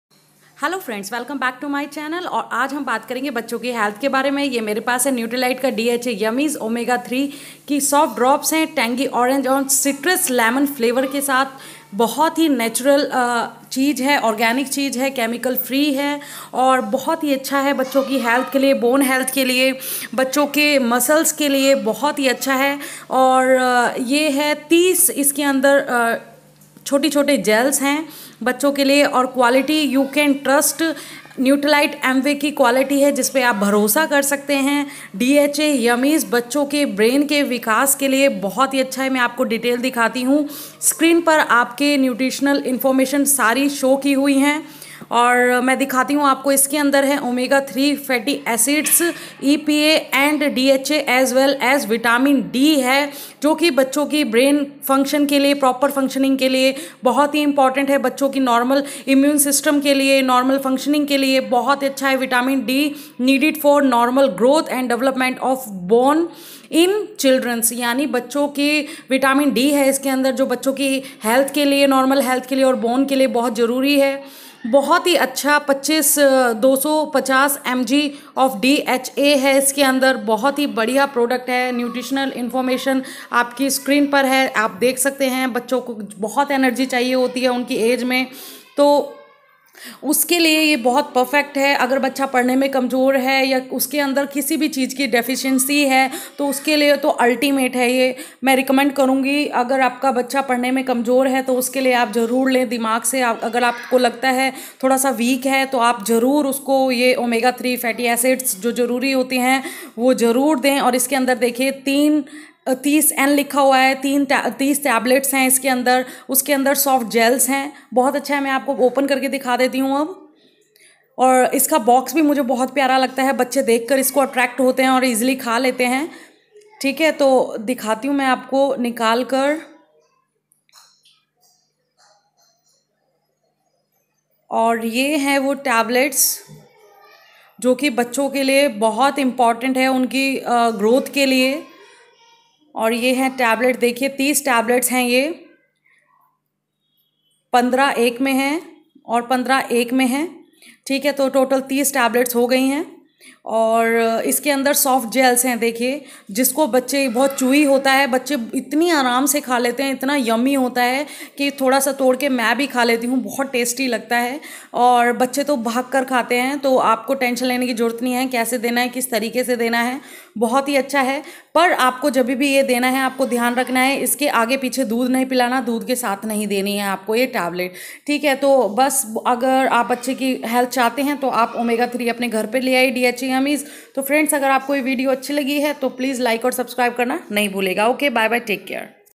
बच्चो के दिमागी और शारीरिक विकास के लिए ज़रूर बच्चो को खिलाए नट्रिलाइट DHA Yummies
DHA Yummies

Amway Nutrilite DHA Yummies Omega 3 soft drop for kids brain health and over all development

DHA as well as Vitamin D. DHA contributes to the maintenance of normal brain function.*

Vitamin D contributes to the normal function of the immune system in children. Vitamin D is needed for normal growth and development of bone

in children.

(*The beneficial effect is obtained with a daily intake of 250 mg of DHA)

Benefits

DHA Yummies contain the Omega-3 fatty acids EPA and DHA as well as Vitamin D. DHA contributes to the maintenance of normal brain function.*

in children.

Use Instructions

Suggested Daily Intake: 1 Soft Drop Per Day

Health Awareness By Shubhangi Verma